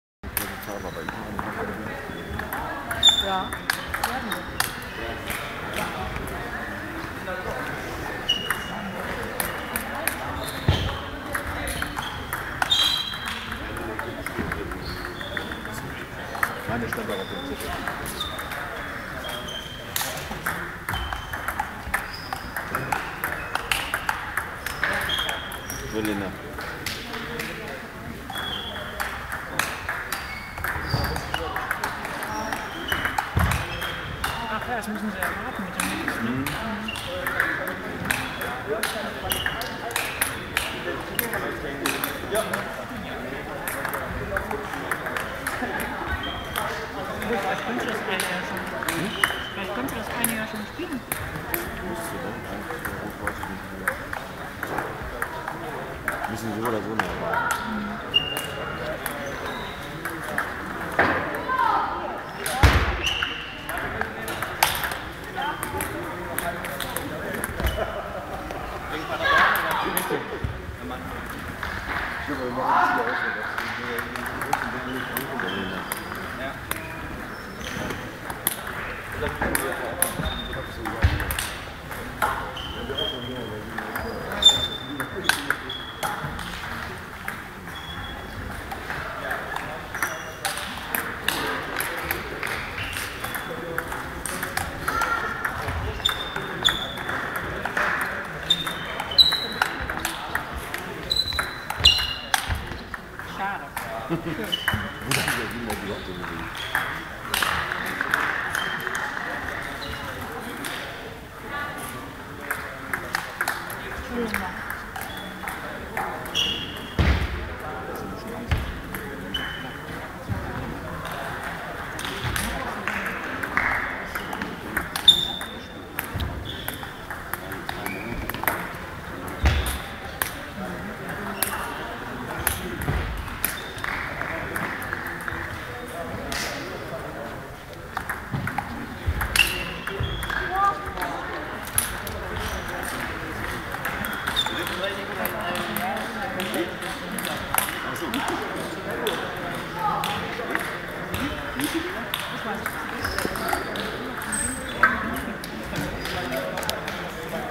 1. TTC Postsportverein Gütersloh Kreisauswahlspiele SchülerInnen A
Am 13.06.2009 fanden bei der DJK Gütersloh die Kreisauswahlspiele des Bezirks OWL statt. Unsere Spielerin Cora Ludwig nahm an Position 2 im Kreisauswahlteam Wiedenbrück teil.